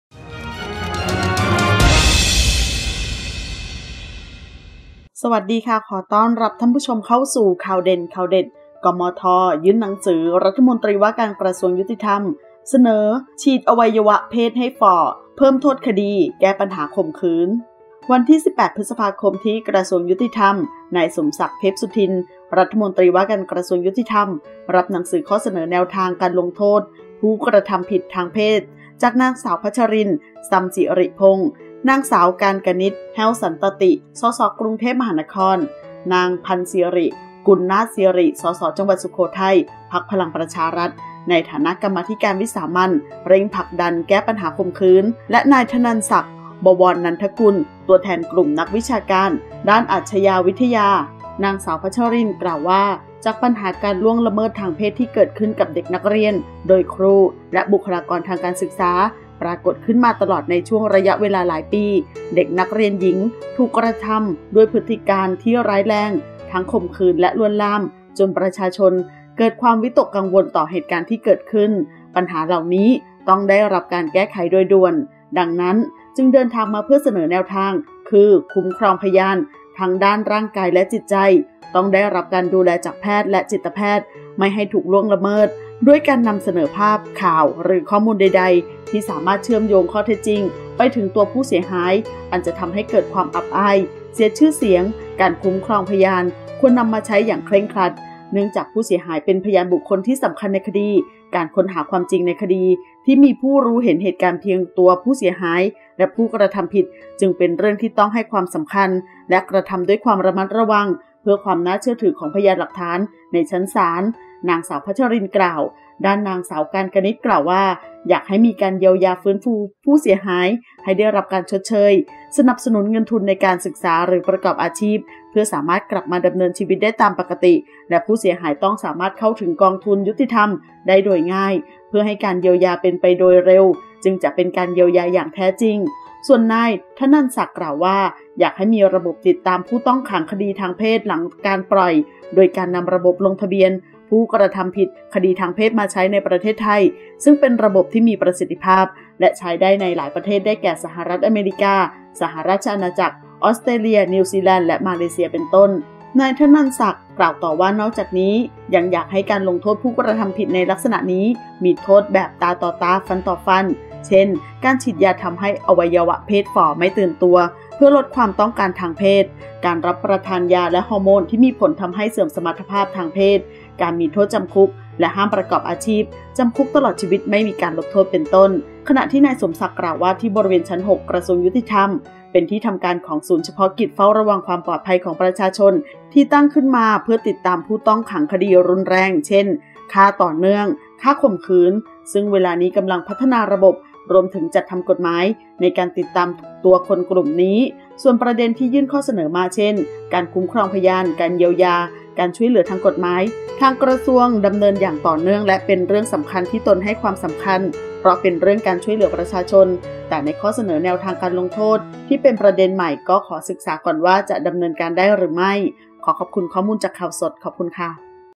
กมธ. ยื่นหนังสือ รมว.ยุติธรรม เสนอฉีดอวัยวะเพศให้ฝ่อ เพิ่มโทษคดี แก้ปัญหาข่มขืน
ขอบคุณที่มา :
ขอขอบคุณข้อมูลจาก: